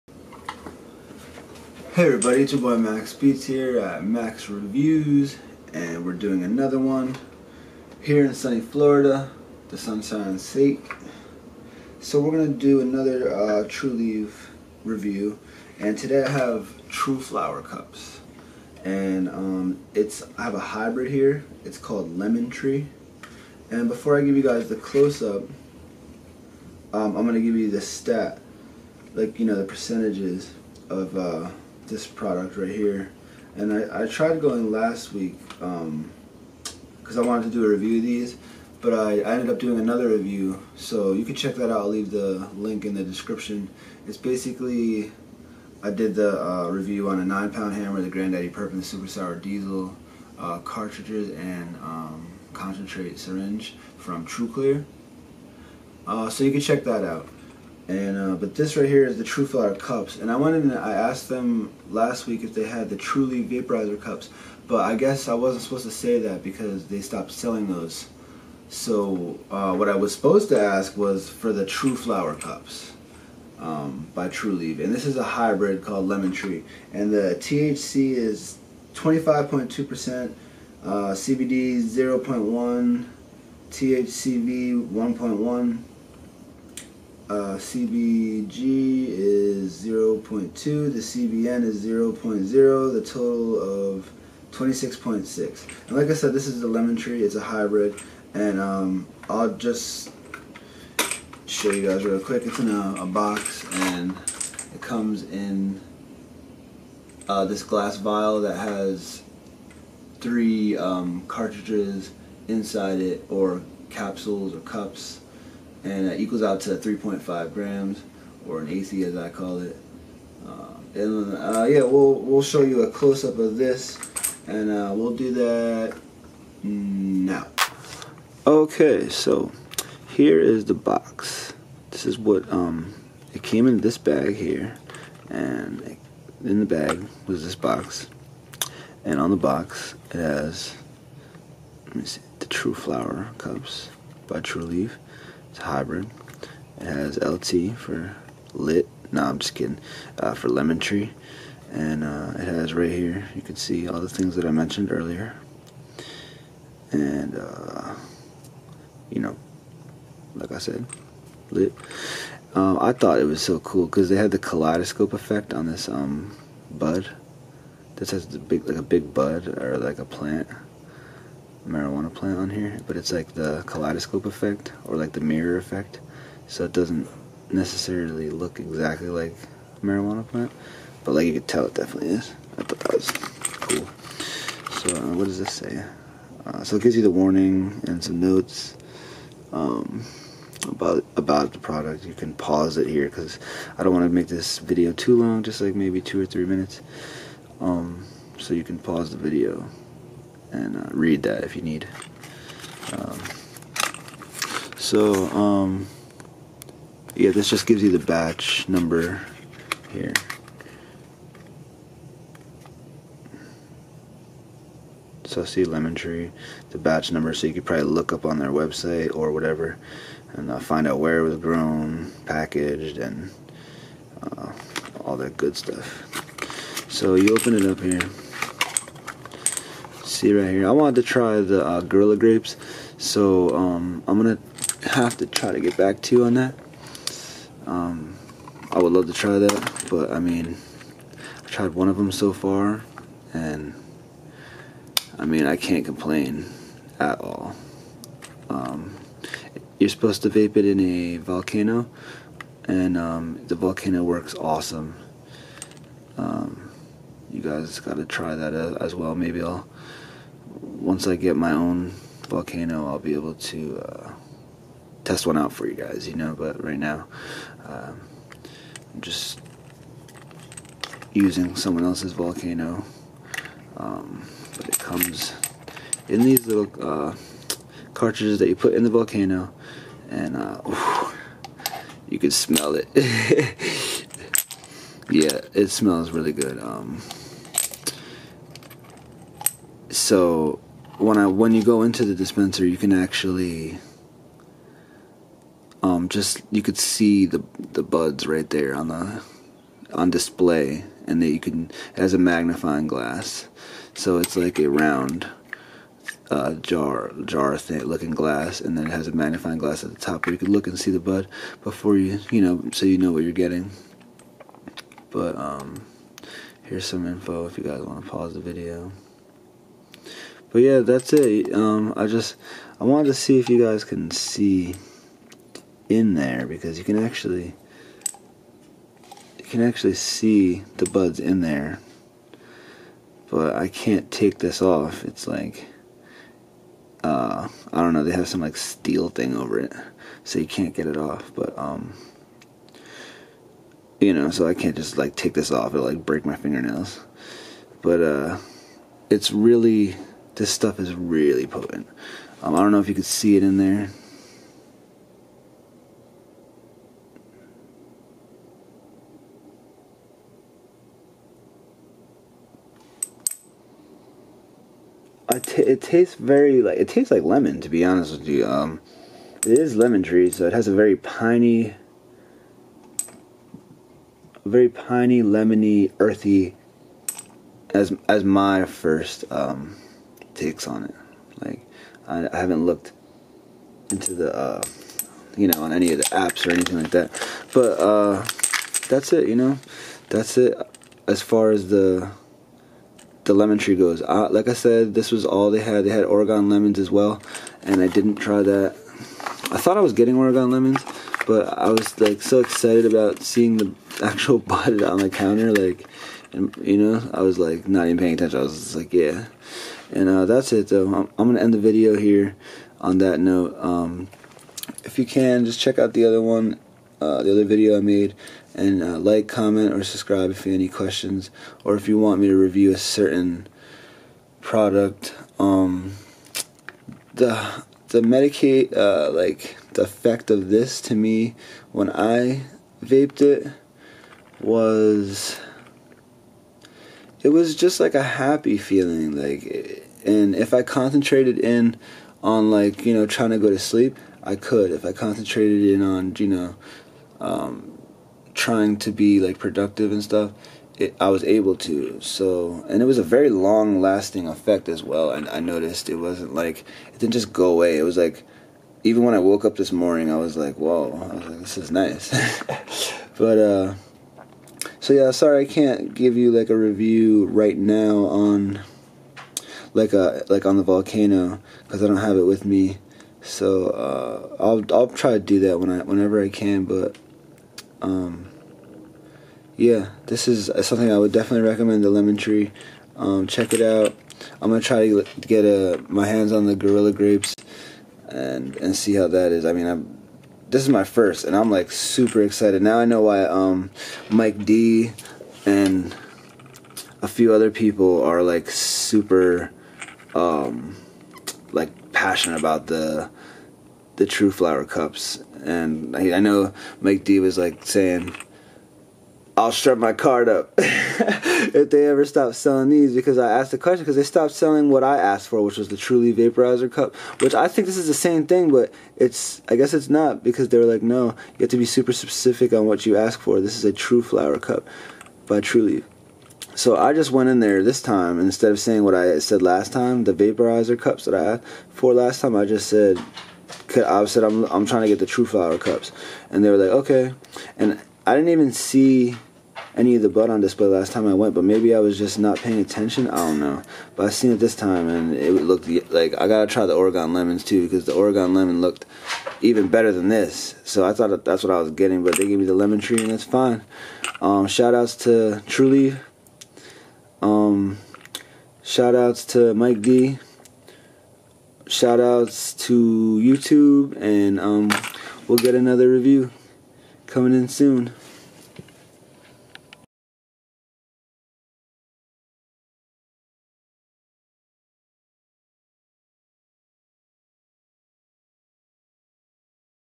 Medical Cannabis TruFlower Cups By Trulieve (Review)
Florida Medical Marijuana Trulieve Tru Flower Review of  Lemon Tree Strain . I recommend this for sure as it was good in my opinion I hope you enjoyed please feel free to like share or subscribe to my channel and thank you again for watching !